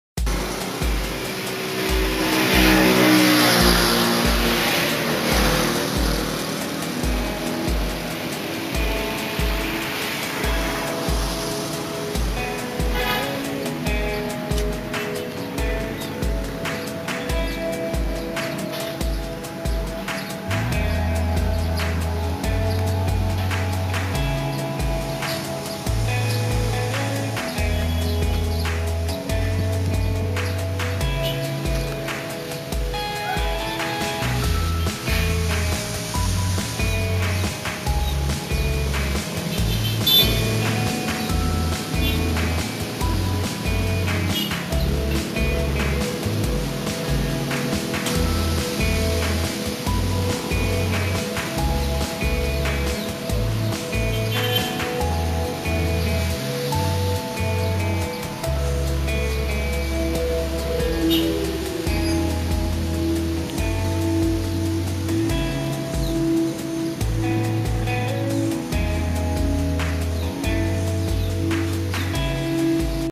Jalan El Tari, New Governor Office Under Construction
Office of Governor of East Nusa Tenggara : Sasando Building. Big wow wow wow...! Super cool!!! The first Sasando Style Building in the world! Lovely!